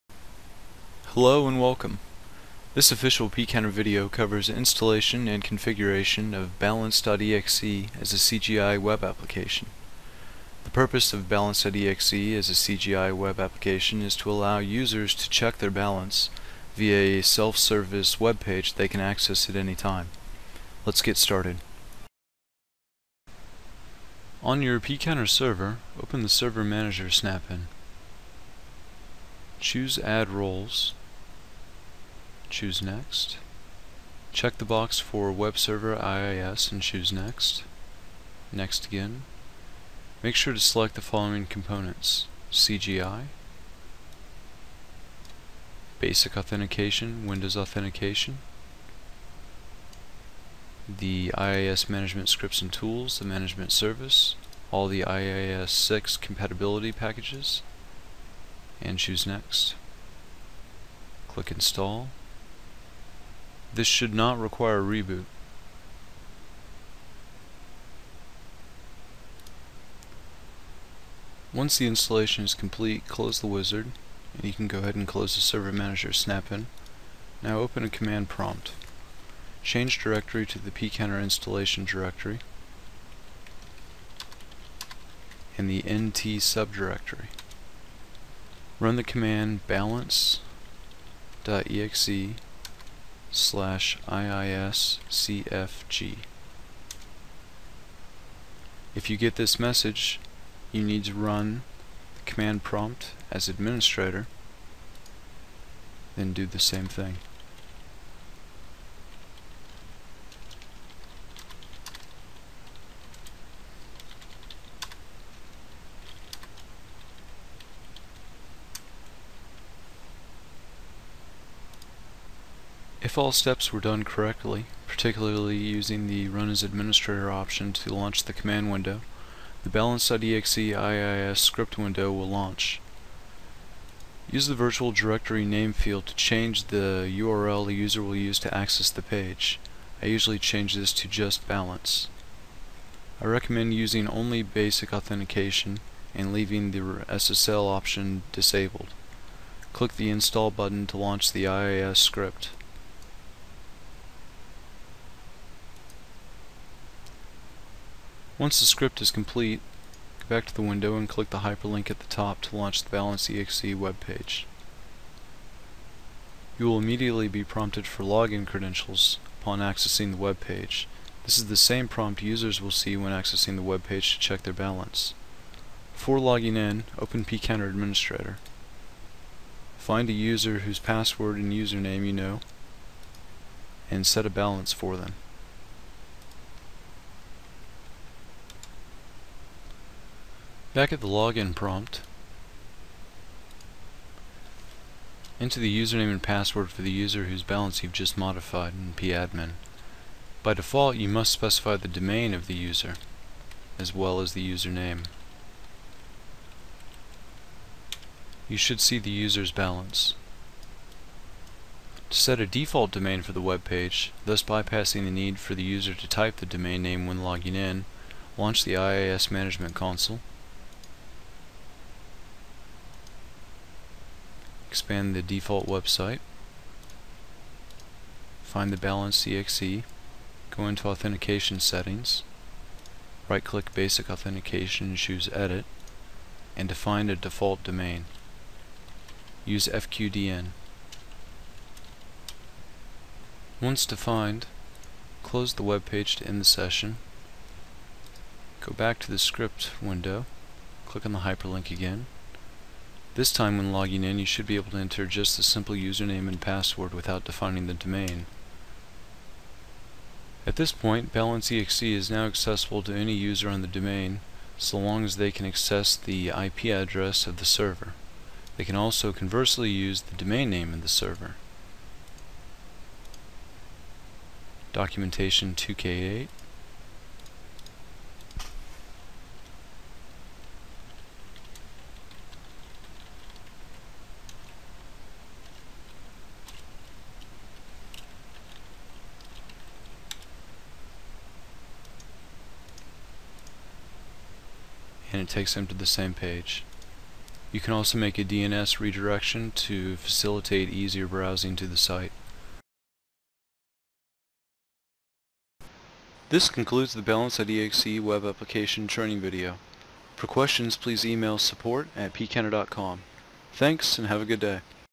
Pcounter for Windows - Balance.exe as a web app - Server 2008 R2
Install and configure Balance.exe as a web application. This provides a web page users can use to check their own balance at any time, using their username and password.